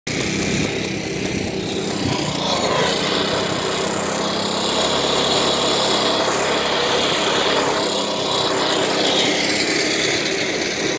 رمي خليط الاسمنت والرمل والحصى لتهييء سقف أحد الطوابق
عملية تهييء سقف المنزل بالخرسانة